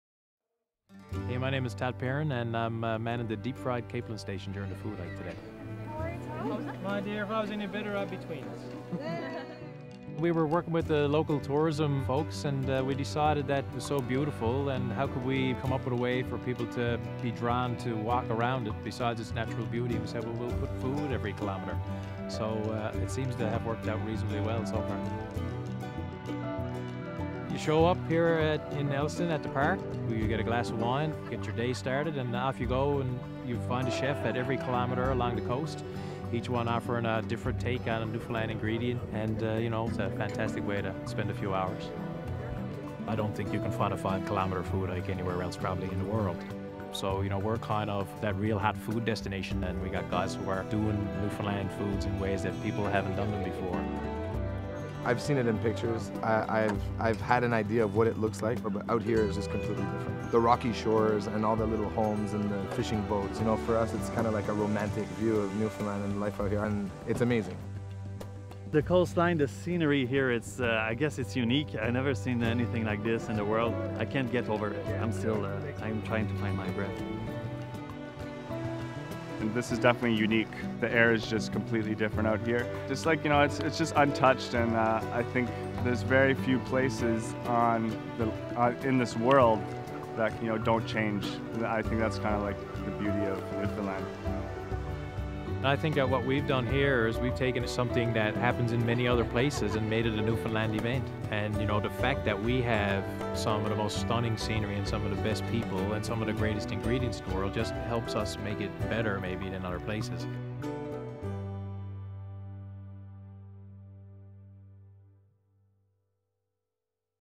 Food on the Move in Newfoundland and Labrador
This annual 5-kilometre food hike begins in Elliston and loops throughout the neighbouring town of Maberly, along winding roads that curve around artful beaches, inlets, coves, and bays. At every kilometre, you'll find some of Canada's best chefs serving a delicious piece of traditional fare. This beautiful natural setting is the perfect place to taste the culinary renaissance that has hit the shores of this province in recent years.